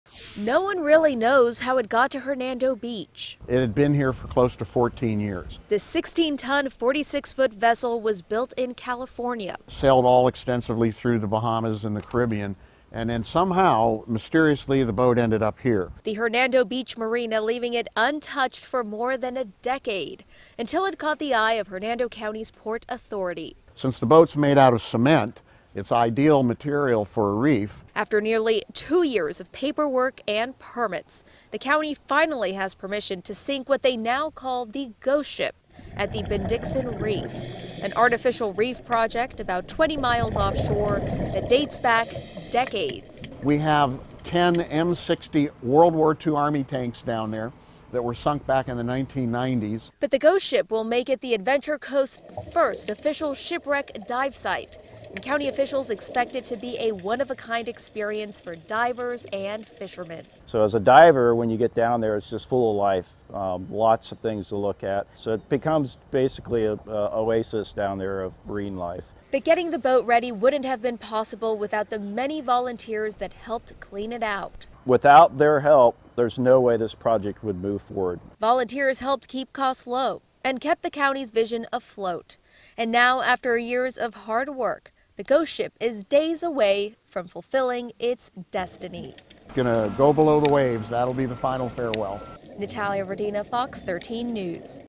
Mystery 'ghost ship' to become part of Adventure Coast artificial reef
A so-called ghost ship will soon sink to the bottom of the ocean off Hernando County to create the Adventure Coasts' first shipwreck dive site.

The 16-ton, 46-foot vessel has sat abandoned at the Hernando Beach Marina for about 14 years. Two years ago, it caught the Hernando County Port Authority's attention.